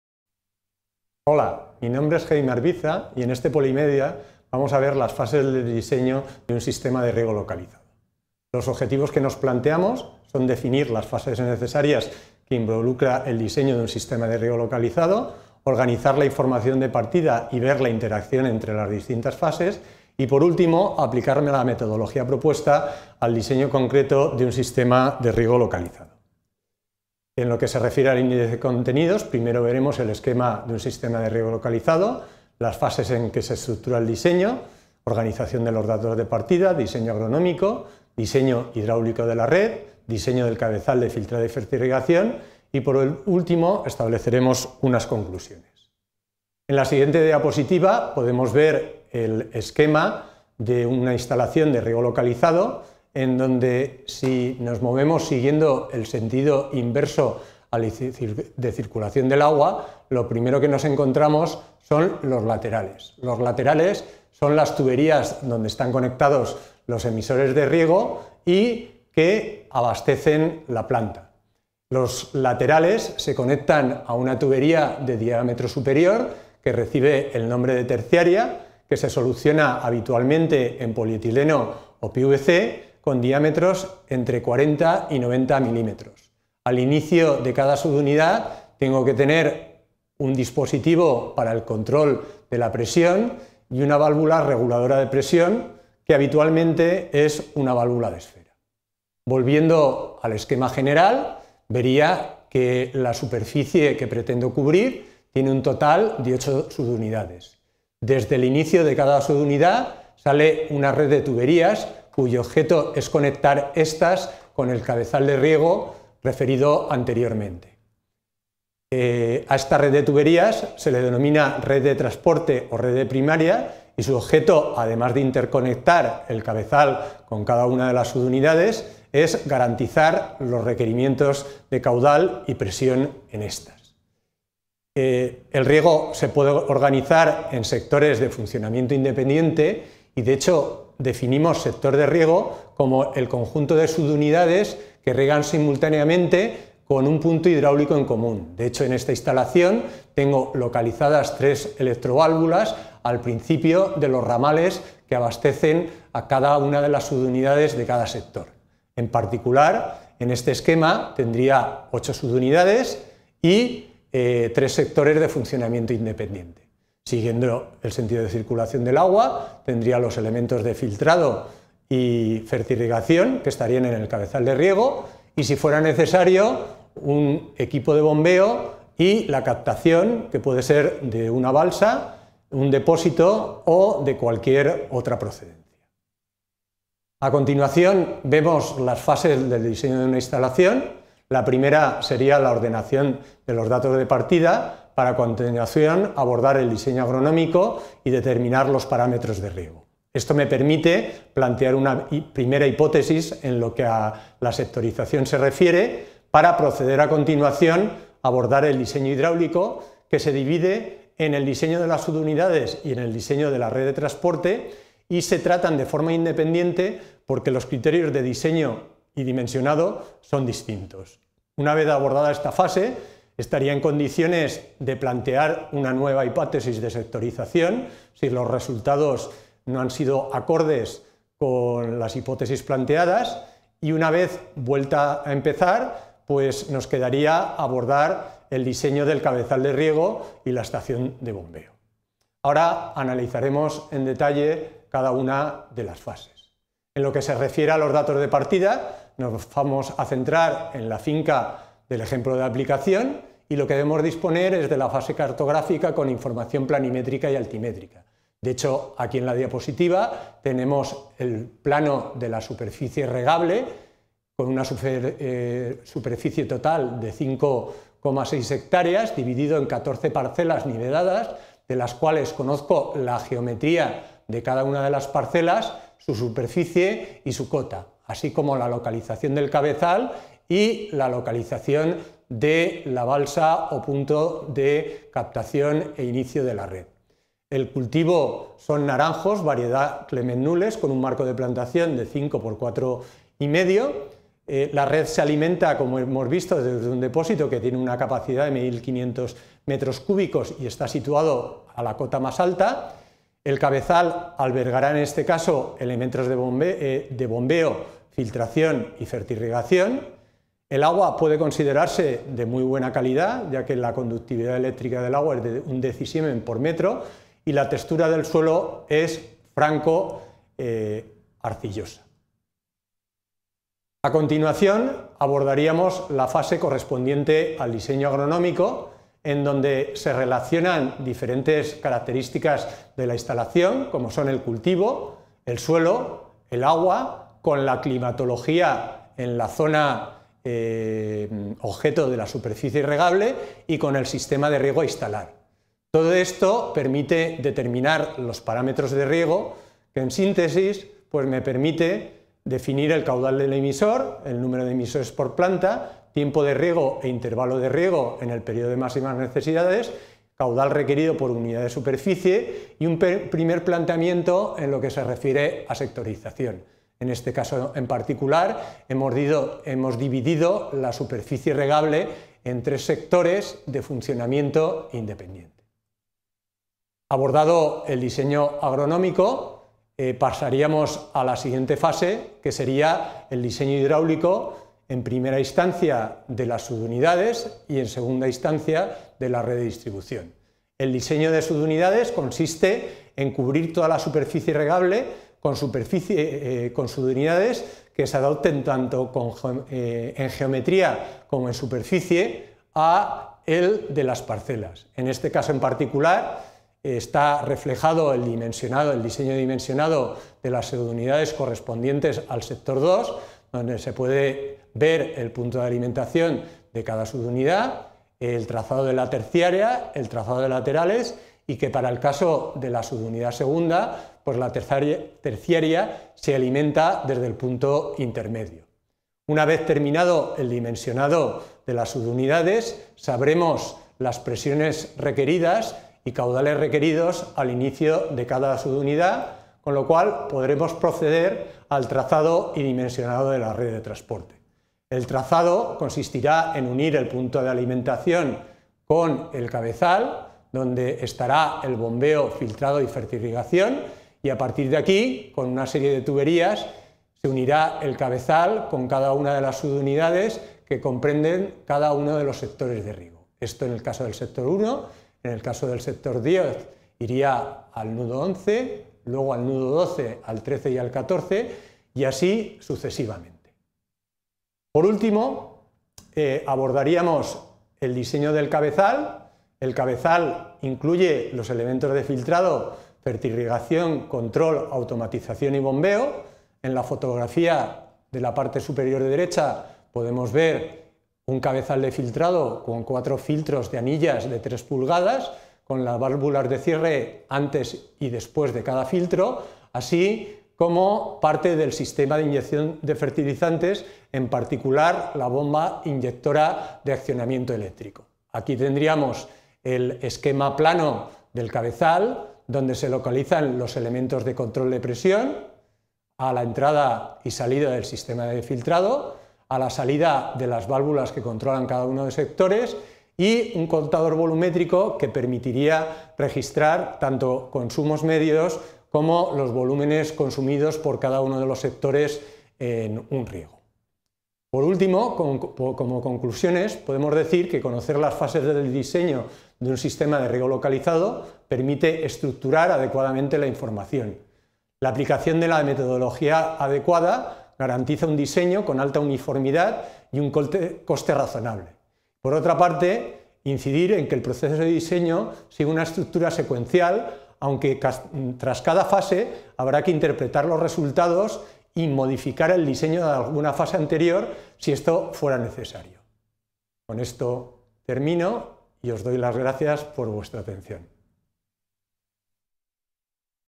Fases del diseño de un sistema de Riego Localizado| UPV
Título: Fases del diseño de un sistema de Riego Localizado

Descripción: En el video se muestran las fases necesarias para abordar el diseño de un sistema de riego localizado, Se expone siguiendo un ejemplo concreto la secuencia de las fases y la interacción entre las mismas. Permite que el alumno tenga clara la metodología y secuencia de actividades que implica el diseño de un sistema de riego localizado. Arviza Valverde, J. (2017). Fases del diseño de un sistema de Riego Localizado.

Autor/a: Arviza Valverde Jaime